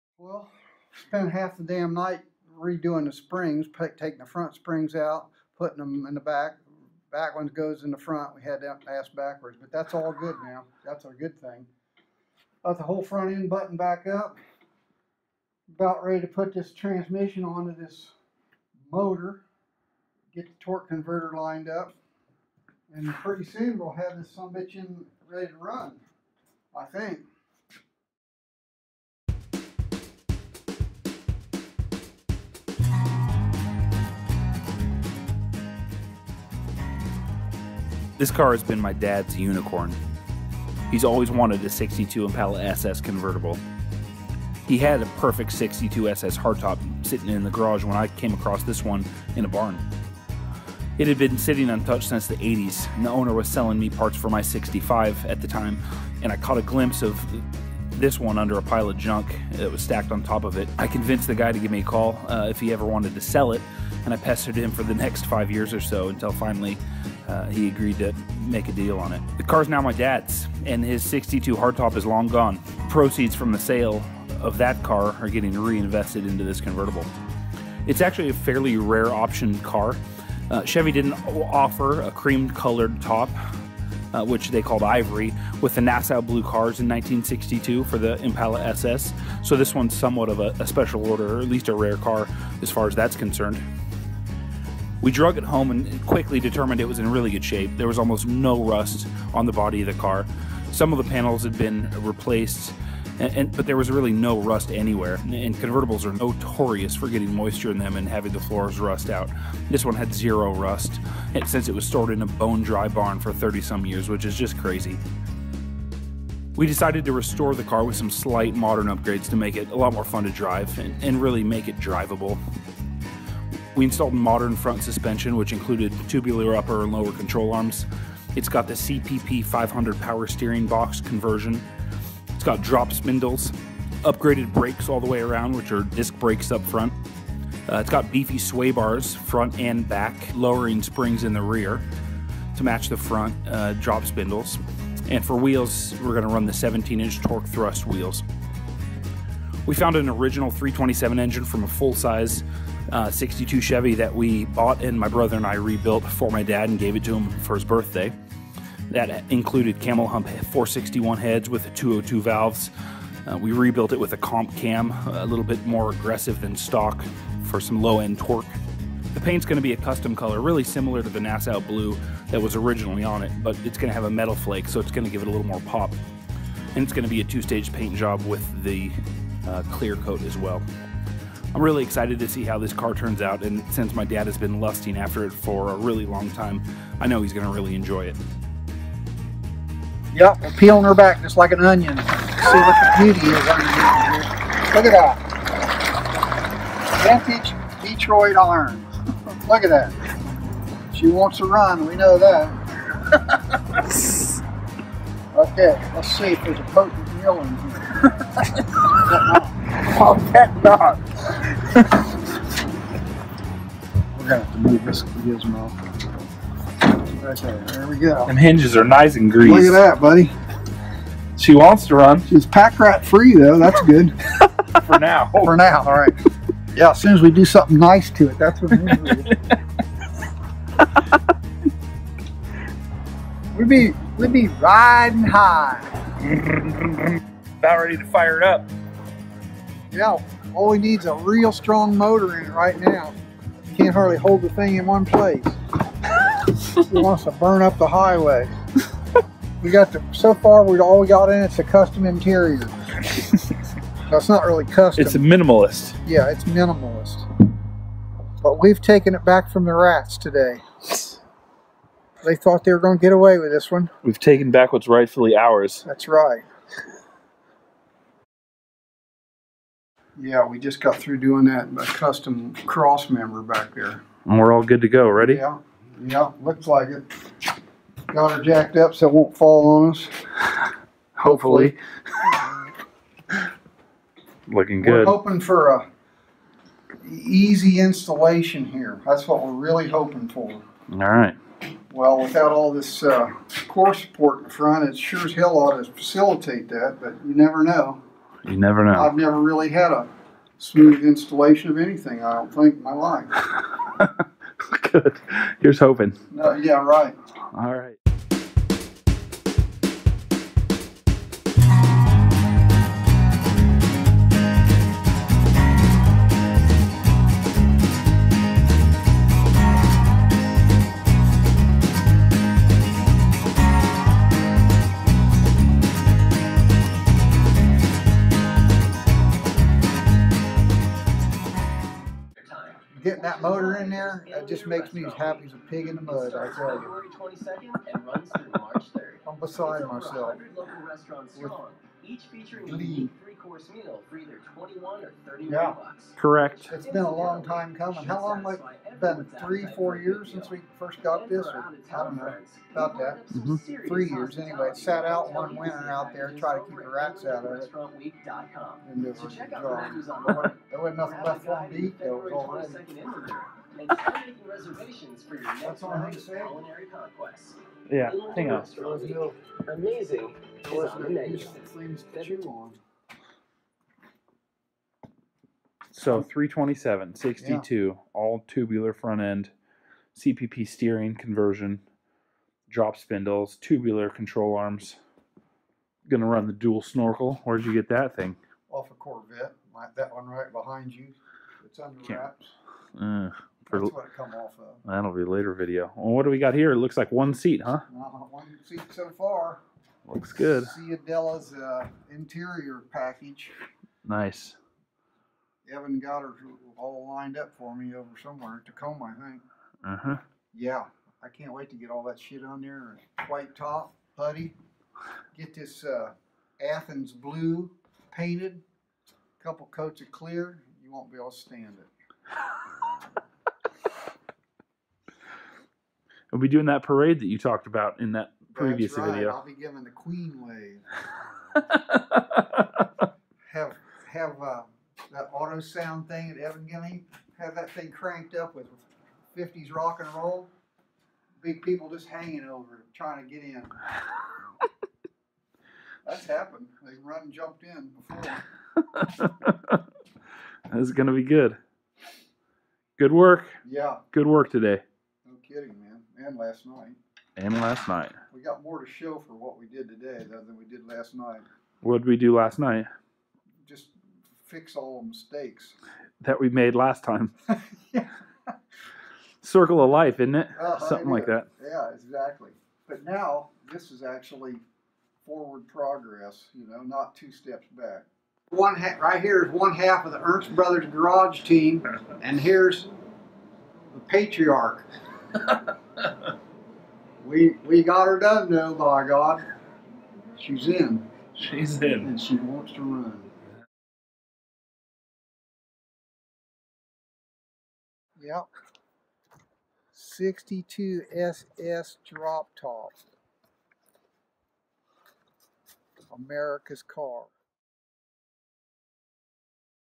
Barn Find: 1962 Impala SS Convertible Project Update
Here's an update on the 1962 Impala SS Convertible Barn Find Project. This is a true 62 Impala SS convertible barn find. The full restoration is underway, and we've got some great hot rod performance upgrades planned.  The Chevy Impala is one of the most recognized classic cars of all Chevrolets. This car is an icon in the automotive industry, and we plan to take her back to her former glory.